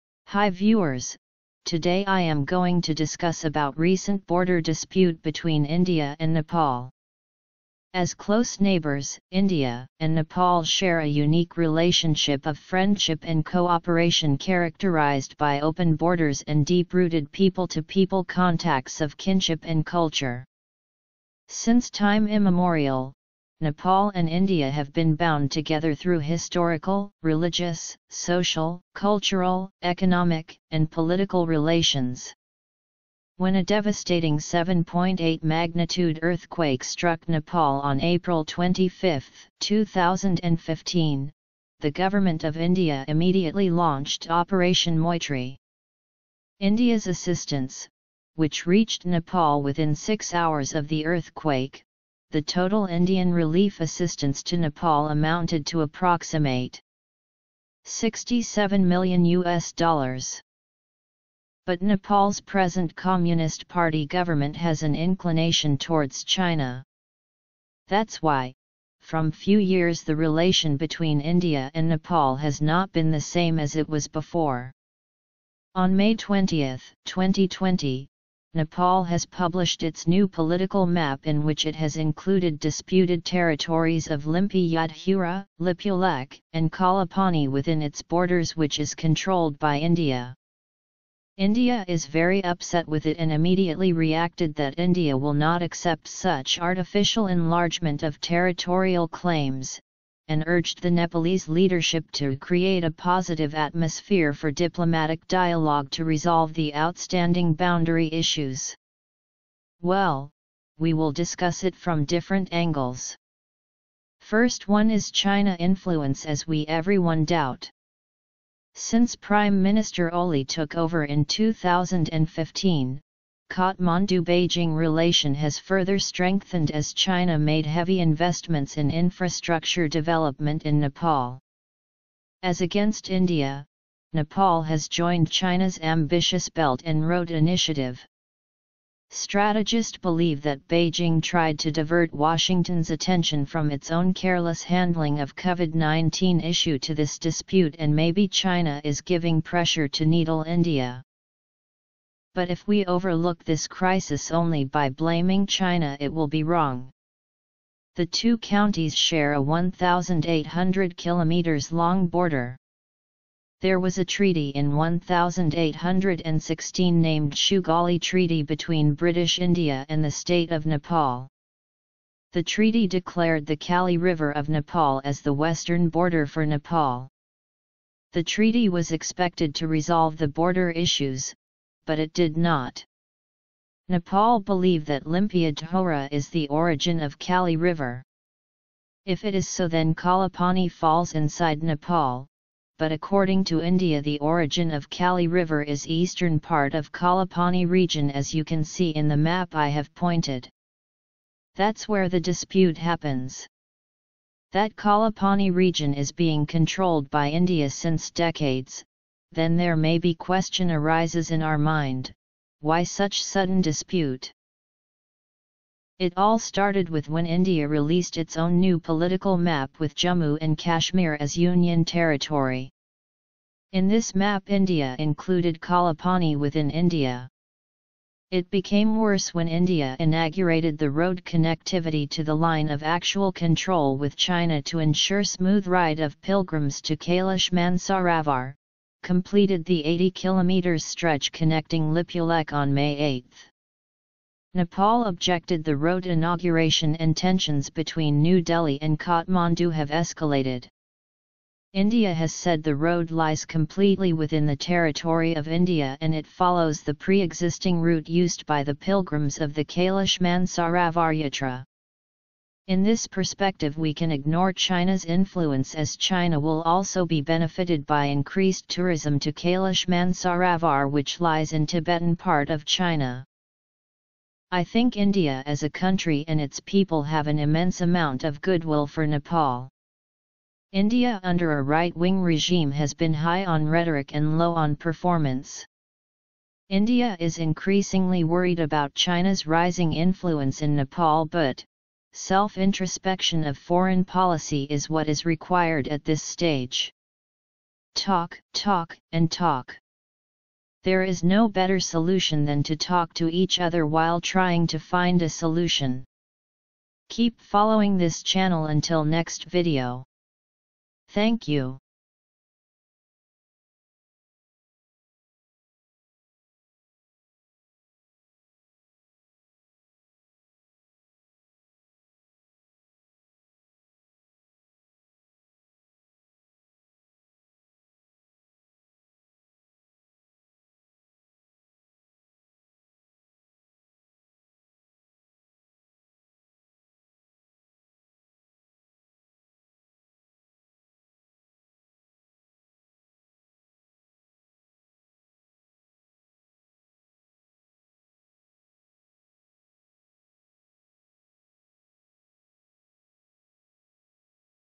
INDIA- NEPAL border Dispute. China's game?? (EXPLAINED)
Hi viewers, I have analyzed different perspective on recent India -Nepal crisis. You can also give your views in the comment section.